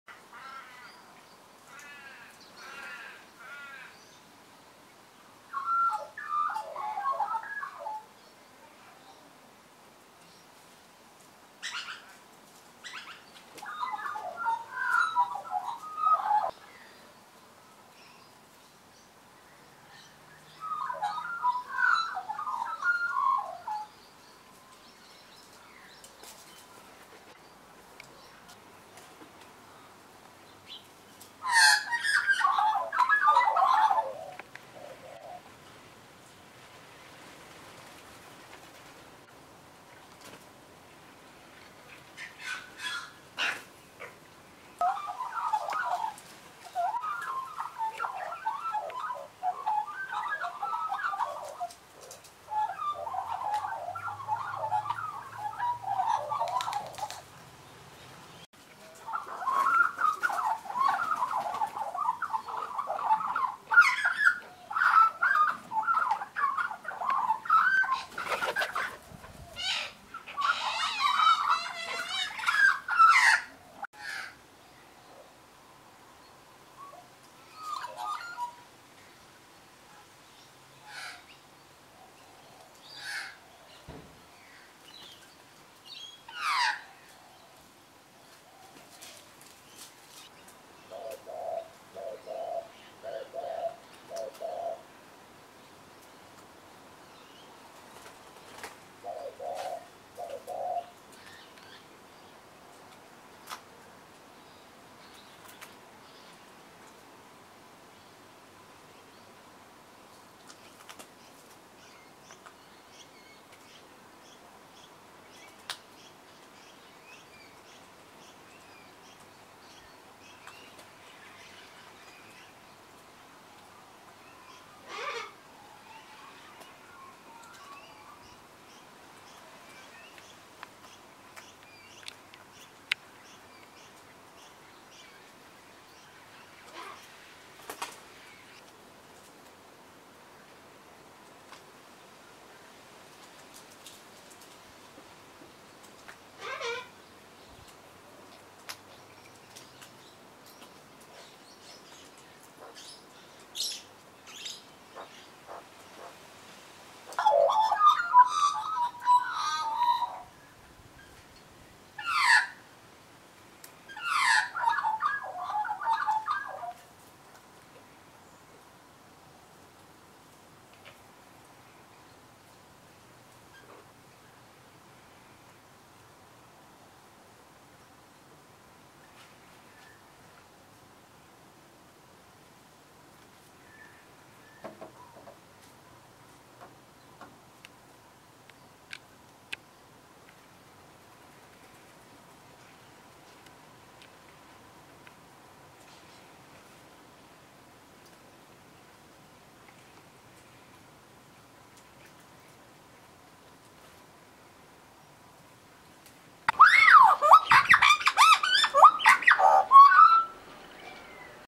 Australian Magpies Hanging Around - Part 1
We remember the first video we ever uploaded of the magpie hanging upside down off a towel on our clothesline We received so many comments from concerned people worried that the magpie was stuck on the towel with his claws, but he was just in fact playing!
Australian Magpies are notorious for this type of display when they are juveniles. It was their favourite game they played in our backyard.